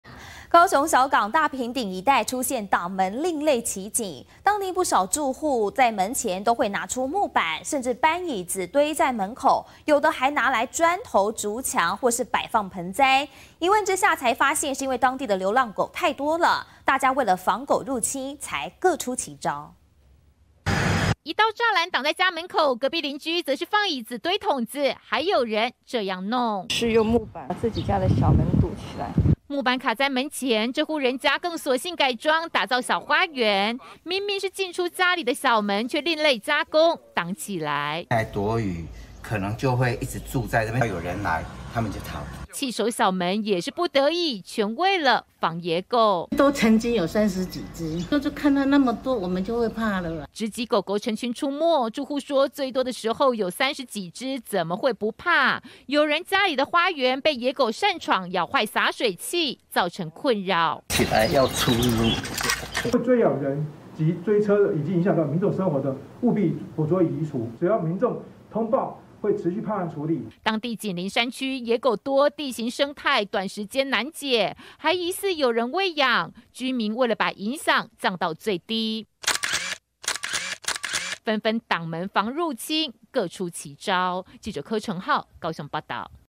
進出家裡小門另類加工 防野狗擅闖入侵
高雄小港大坪頂一帶，出現擋門奇景！當地不少住戶，棄守小門，有用木板堵住、也有搬椅子堆門口，甚至磚頭築牆的，一問才知道，當地野狗不少，為了防狗入侵，各出其招。